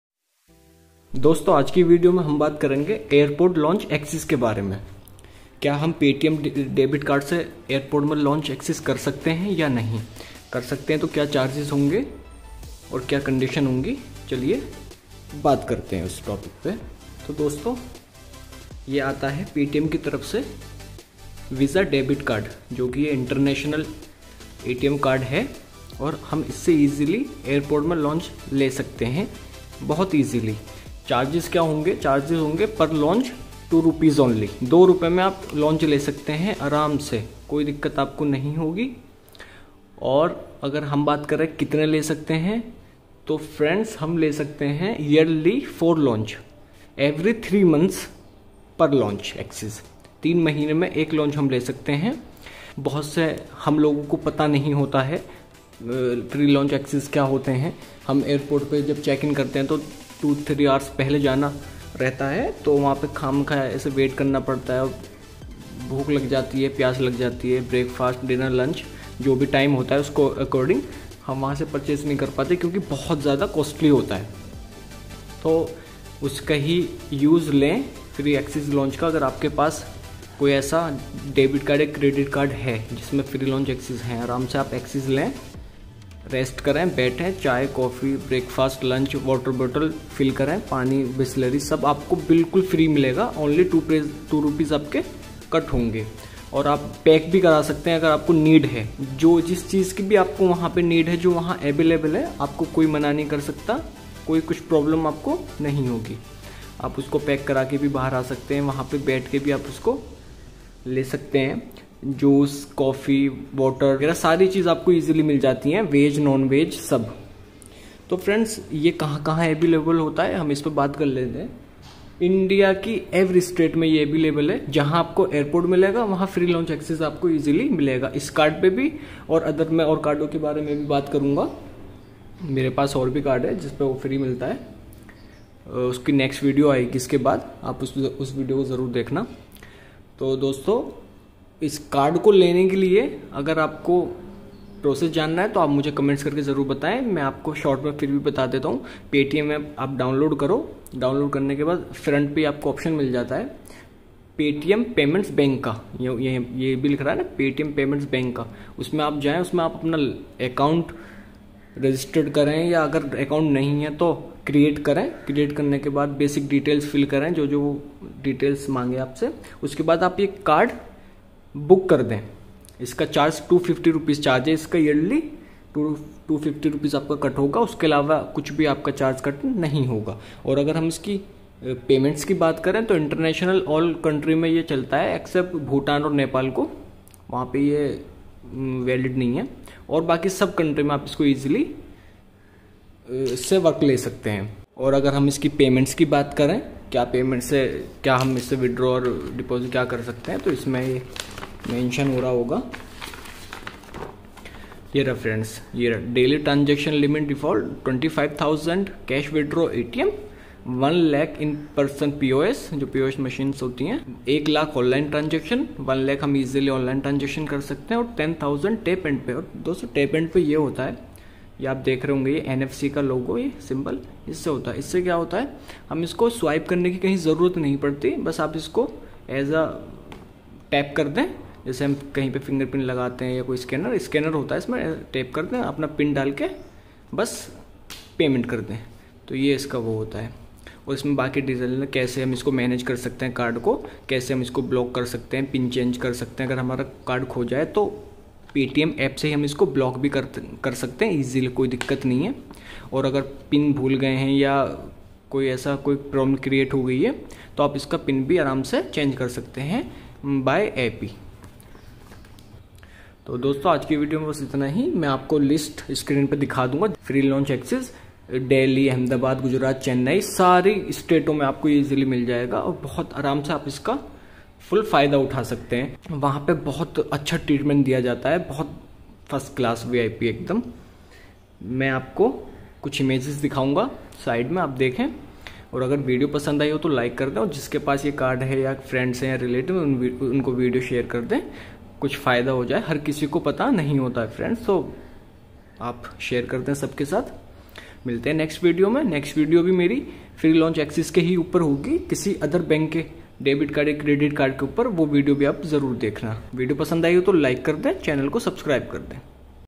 Paytm Debit Card Free Airport Lounge Access | Only ₹2 | Paytm Visa ATM Card Free Lounge Airport
Hello Dosto, is video mein maine aapse baat ki hai Paytm Visa Debit Card ke baare mein

Aur share ki hai Uski Kuch Advantage Jaise Ki Airport Main Free Lounge Kaise Le Sakte Hein Or Kitne Le Sakte Hein Yearly 4 Lounge Access Le Sakte Hai

Product details - Paytm Visa Debit Card By Paytm Payments Bank

Mujhe Umeed hai Aapko Video Pasand aayi hogi